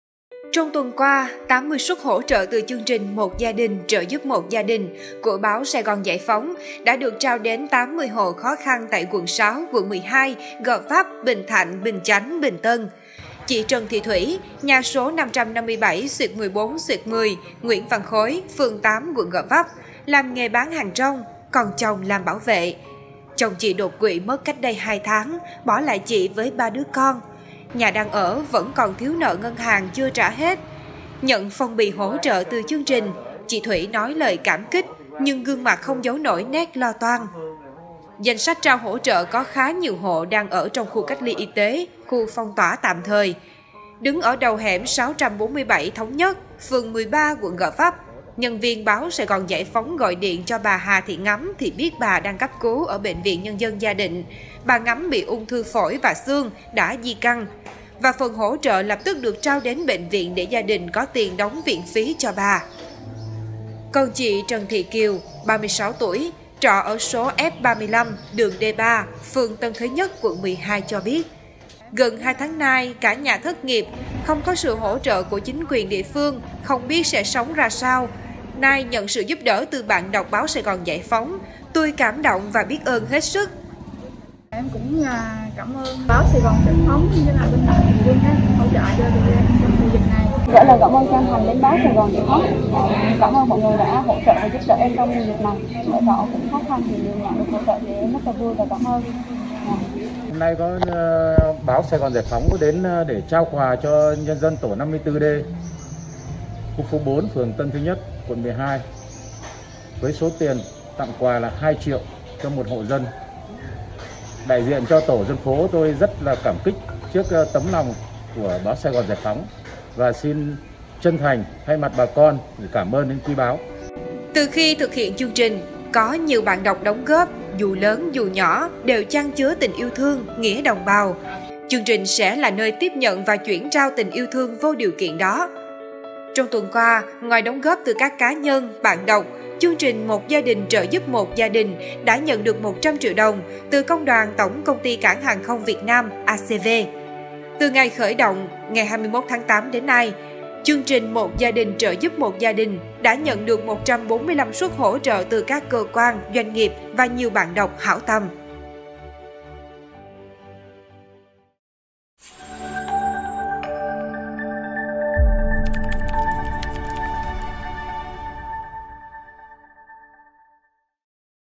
Sẻ chia yêu thương kịp lúc
Trong tuần qua, 80 suất hỗ trợ từ chương trình “Một gia đình trợ giúp một gia đình” của Báo SGGP đã được trao đến 80 hộ khó khăn tại quận 6, quận 12, Gò Vấp, Bình Thạnh, Bình Chánh, Bình Tân. Từ khi thực hiện chương trình, có nhiều bạn đọc đóng góp, dù lớn, dù nhỏ, đều chan chứa tình yêu thương, nghĩa đồng bào. Chương trình sẽ là nơi tiếp nhận và chuyển trao tình yêu thương vô điều kiện đó.